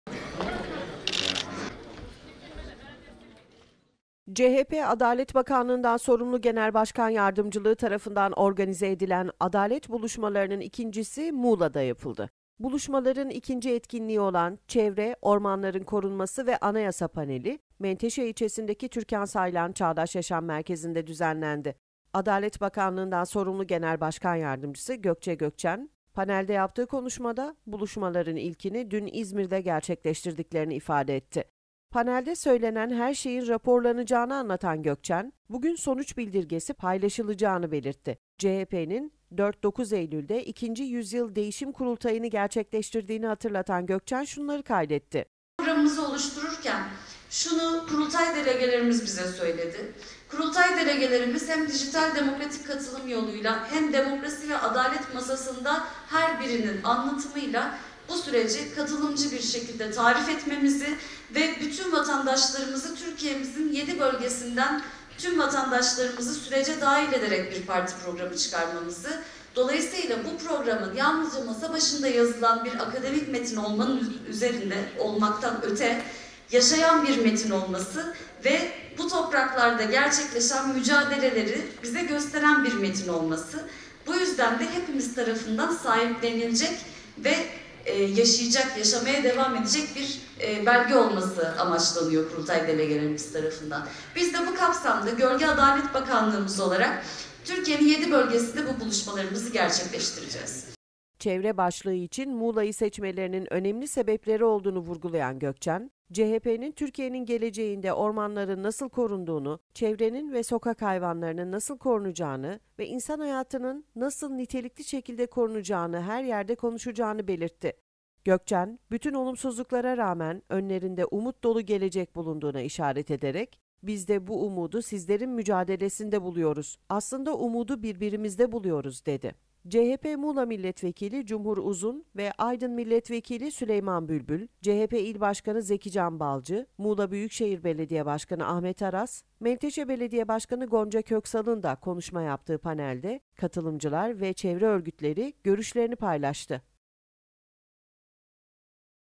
ADALET BULUŞMALARI NIN İKİNCİSİNİ MUĞLA'DA DÜZENLEDİ
CHP "Adalet Buluşmaları"nın ikincisini Muğla'da düzenledi

CHP Adalet Bakanlığından Sorumlu Genel Başkan Yardımcılığı tarafından organize edilen "Adalet Buluşmaları"nın ikincisi Muğla'da yapıldı.

Buluşmaların ikinci etkinliği olan " Çevre, Ormanların Korunması ve Anayasa" paneli, Menteşe ilçesindeki Türkan Saylan Çağdaş Yaşam Merkezi'nde düzenlendi.

Adalet Bakanlığından Sorumlu Genel Başkan Yardımcısı Gökçe Gökçen, panelde yaptığı konuşmada, buluşmaların ilkini dün İzmir'de gerçekleştirdiklerini ifade etti.

Panelde söylenen her şeyin raporlanacağını anlatan Gökçen, bugün sonuç bildirgesi paylaşılacağını belirtti. CHP'nin 4-9 Eylül'de 2. Yüzyıl Değişim Kurultayı'nı gerçekleştirdiğini hatırlatan Gökçen, şunları kaydetti:

"Çevre" başlığı için Muğla'yı seçmelerinin önemli sebepleri olduğunu vurgulayan Gökçen, CHP'nin Türkiye'nin geleceğinde ormanların nasıl korunduğunu, çevrenin ve sokak hayvanlarının nasıl korunacağını ve insan hayatının nasıl nitelikli şekilde korunacağını her yerde konuşacağını belirtti. Gökçen, bütün olumsuzluklara rağmen önlerinde umut dolu gelecek bulunduğuna işaret ederek, "Bizde bu umudu sizlerin mücadelesinde buluyoruz. Aslında umudu birbirimizde buluyoruz." dedi.

CHP Muğla Milletvekili Cumhur Uzun ve Aydın Milletvekili Süleyman Bülbül, CHP İl Başkanı Zekican Balcı, Muğla Büyükşehir Belediye Başkanı Ahmet Aras, Menteşe Belediye Başkanı Gonca Köksal'ın da konuşma yaptığı panelde, katılımcılar ve çevre örgütleri görüşlerini paylaştı.